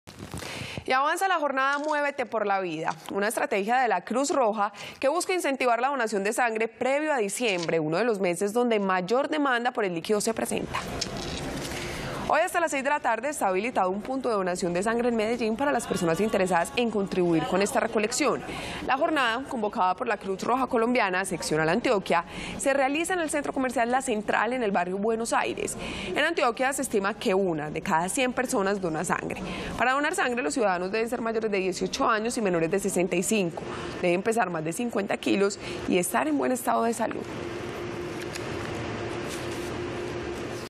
La Cruz Roja realizó nueva jornada de Muévete por la Vida [Noticias] - Telemedellín
Avanza la jornada Muévete por la Vida, una estrategia de la Cruz Roja que busca incentivar la donación de sangre previo a diciembre, uno de los meses donde mayor demanda por el líquido se presenta.